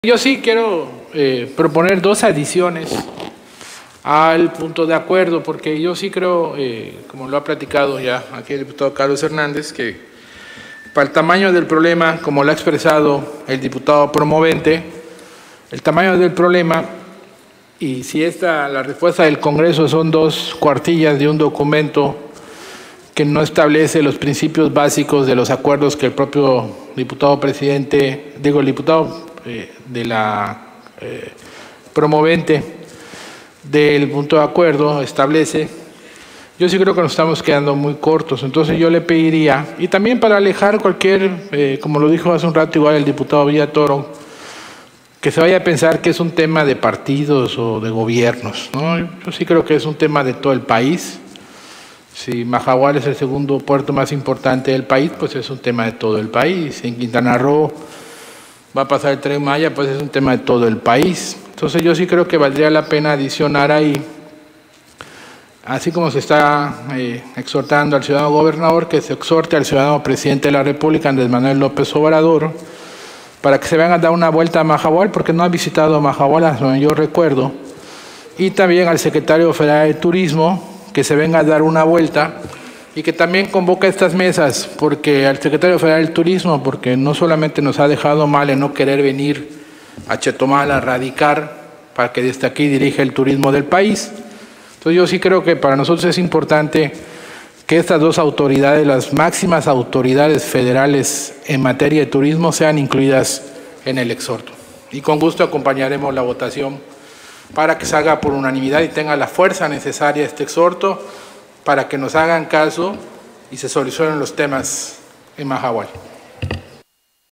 Intervención del Dip. Eduardo Martínez Arcila. PAN (23/Nov/2021)
Intervención del Diputado Eduardo Martínez Arcila, Presidente de la Junta de Gobierno y Coordinación Política de la XVI Legislatura, en relación al Acuerdo de obvia y urgente resolución por el que los Diputados Integrantes de la XVI Legislatura Constitucional del Poder Legislativo del Estado Libre y Soberano de Quintana Roo hacemos un atento y respetuoso exhorto al Contador Público Carlos Manuel Joaquín González, Gobernador Constitucional del Estado de Quintana Roo, C. Jorge Arturo Contreras Castillo, Secretario de Gobierno; al encargado de despacho Andrés Aguilar Becerril de la Secretaría de Turismo; Lic. Jorge Pérez y Pérez, Director del Instituto de Movilidad de Quintana Roo, y a la C. Yensunni Idalia Martínez Hernández, Presidenta Municipal del H. Ayuntamiento de Othón P. Blanco, para que, en el ámbito de sus atribuciones y facultades se establezcan convenios entre los propietarios del Muelle del Puerto Costa Maya de Mahahual, el Gobierno del Estado de Quintana Roo, el Instituto de Movilidad del Estado de Quintana Roo, el H. Ayuntamiento de Othón P. Blanco y los distintos prestadores de servicios del poblado de Mahahual, transportistas, hoteleros y restauranteros, así como otros prestadores de servicios de la zona; esto con la finalidad de impulsar una economía leal y en igualdad de condiciones para impulsar la riqueza cultural, turística y natural de la zona; presentado por el Diputado Roberto Erales Jiménez, Presidente de la Comisión de Puntos Legislativos y Técnica Parlamentaria de la XVI Legislatura del Estado.